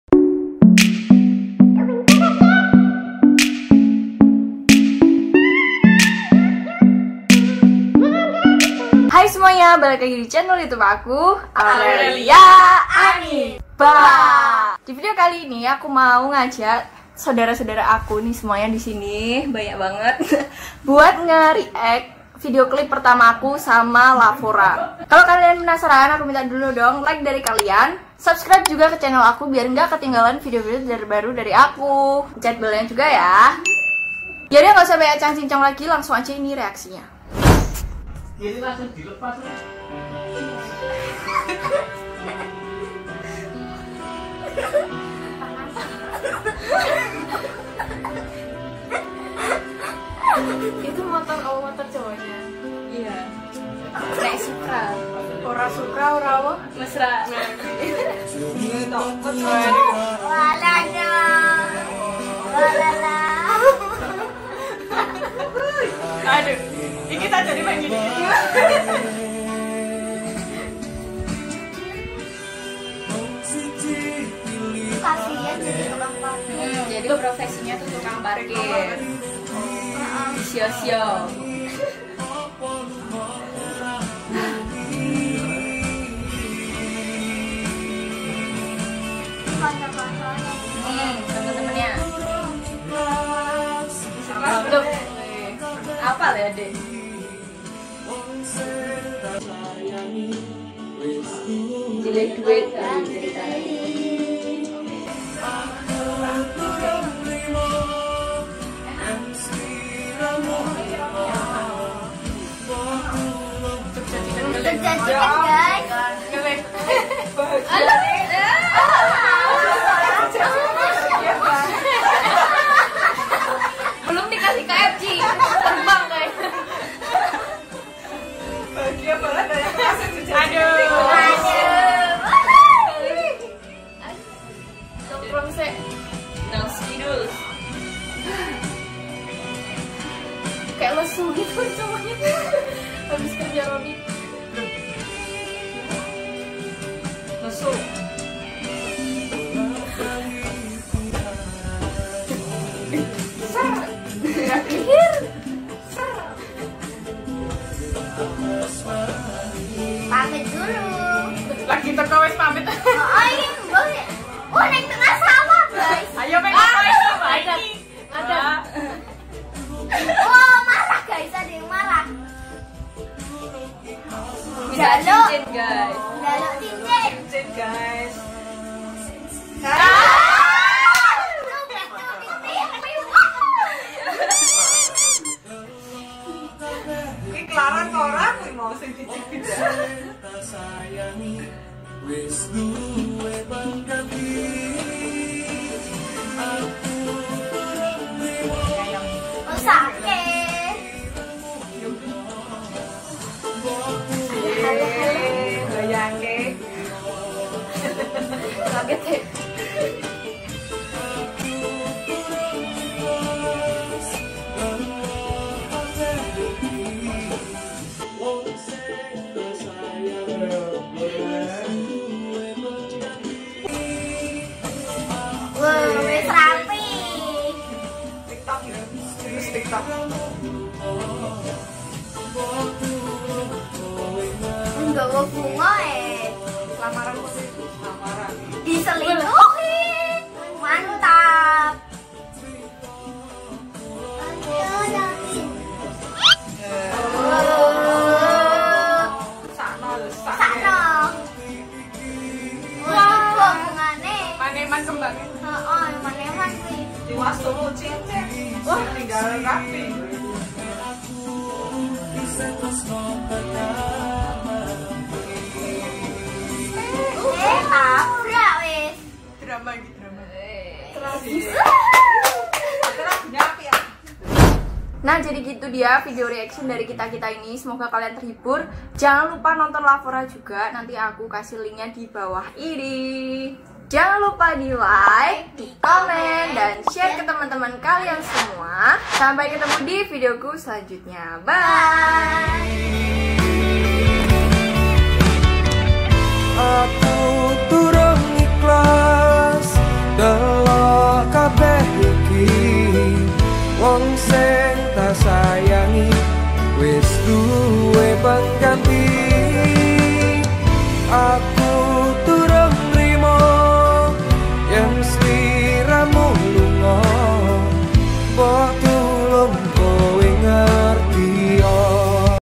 MY FAMILY REACT TO MY FIRST VIDEO CLIP COLLABORATION
HAII GUYSSS!!
Aku habis kolaborasi sama LAVORA untuk bikin video clip. Ini pertama kalinya dalam hidupku HAHAHHAHAAHAHAH.
Jadi aku iseng2 ni bikin video
Kalau menurut kalian gimanaa?

Yuk nonton juga single kedua LAVORA di bawah inii

Aku menerima segala kritik, saran, dan masukan yaa!
Buat kalian yang mau req video langsung komen aja atau kalau malu bisa DM instagram aku aurelliaanin.

See you on the my next video, guys! Ily!